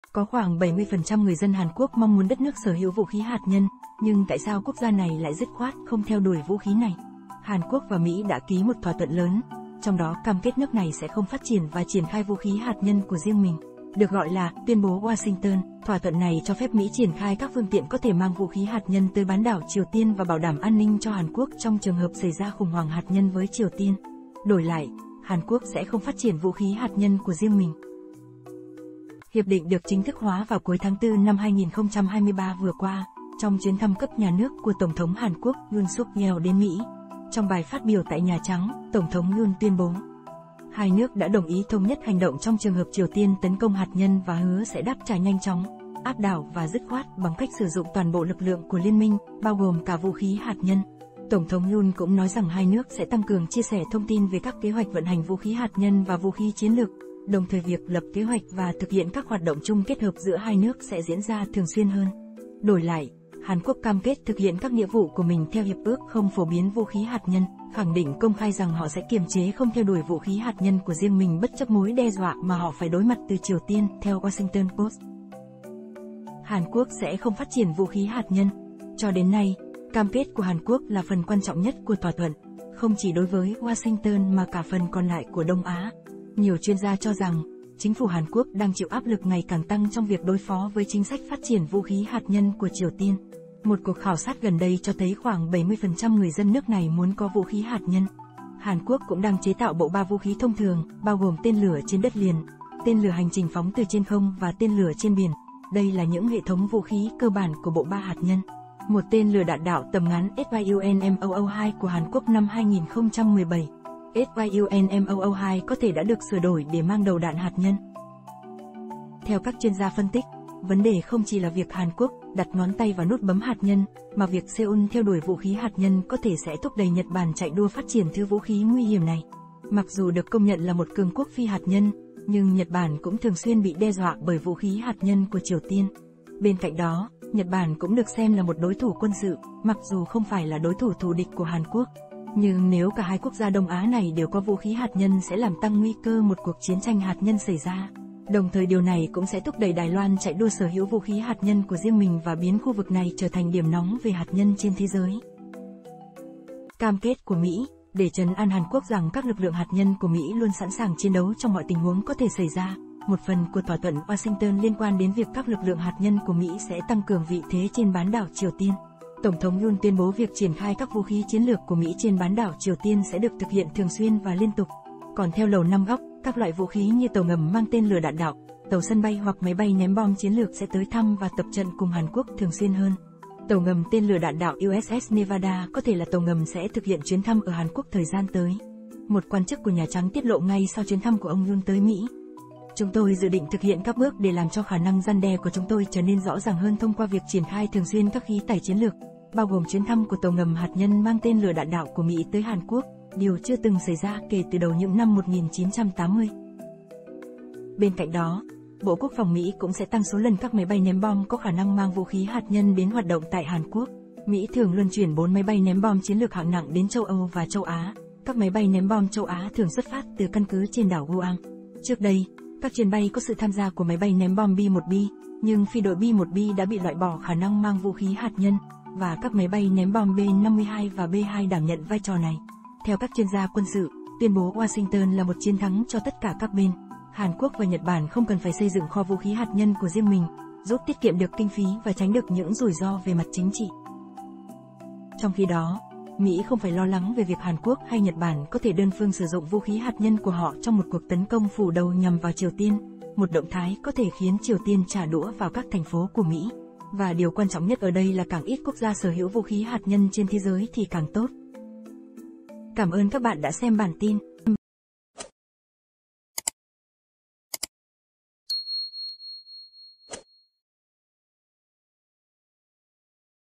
LÝ DO HÀN QUỐC KHÔNG CHẾ TẠO VŨ KHÍ HẠT NHÂN NHƯ TRIỀU TIÊN MÀ KÝ TUYÊN BỐ WASHINGTON
Hàn Quốc và Mỹ đã ký một thỏa thuận lớn, trong đó cam kết nước này sẽ không phát triển và triển khai vũ khí hạt nhân của riêng mình. Được gọi là "Tuyên bố Washington",